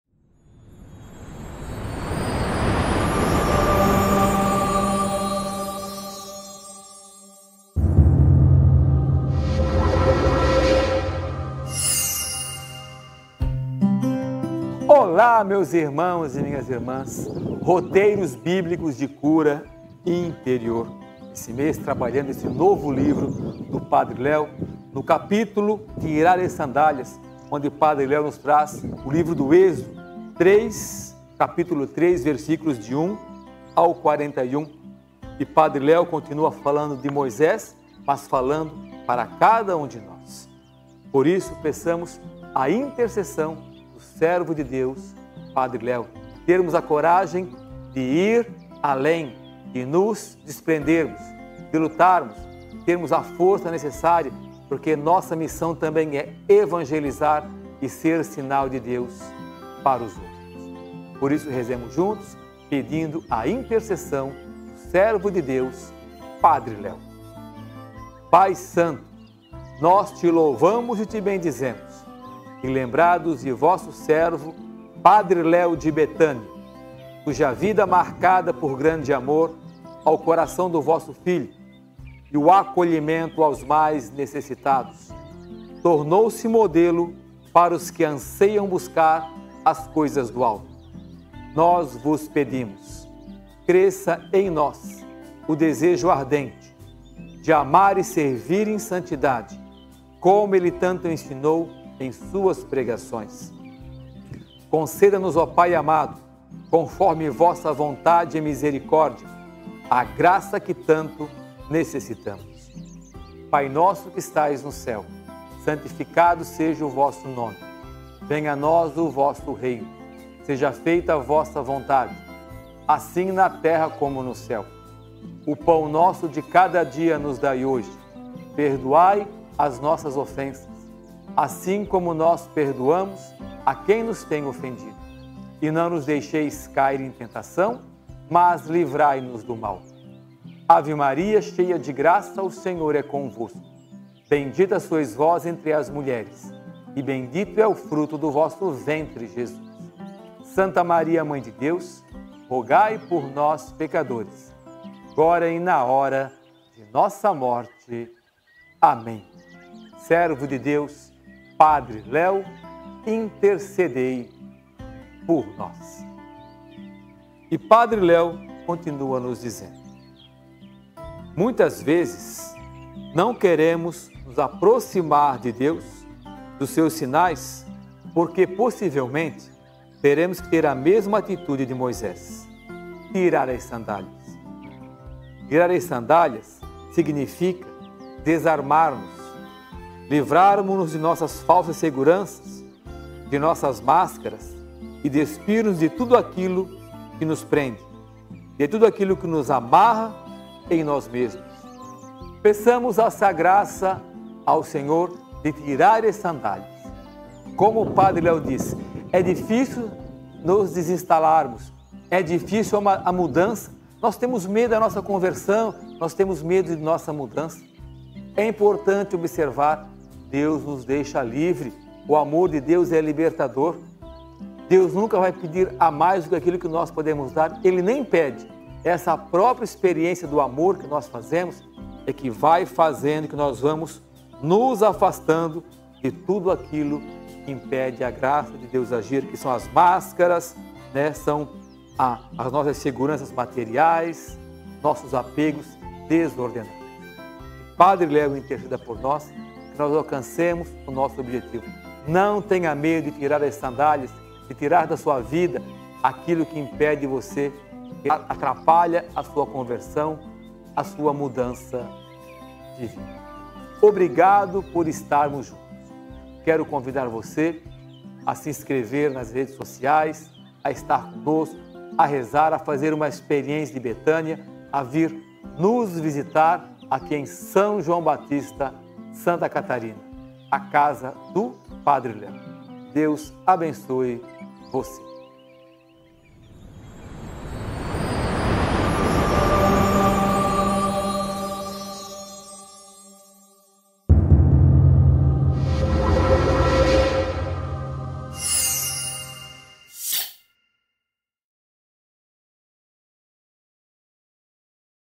Tirar a mascara e buscar a conversão - Padre Léo
Livro do Padre Léo - Roteiros Bíblicos de Cura Interior

Diácono Ideraldo Paloschi, Cofundador da Comunidade Bethânia, tem uma reflexão sobre o livro, Roteiros Bíblicos de Cura Interior

O livro de autoria de Padre Léo  🕯️

Padre Léo diz: 💬

 “Existem momentos de nossa vida em que nosso coração está tão ferido e machucado que nem conseguimos rezar. São momentos de dor, de abandono, de decepção e tristeza tão marcantes que nos impedem de verbalizar uma oração.
                     Foi pensando nisso que resolvi unir neste livro duas de minhas maiores paixões: Cura interior e Bíblia ”

Comunidade Bethânia - Padre Léo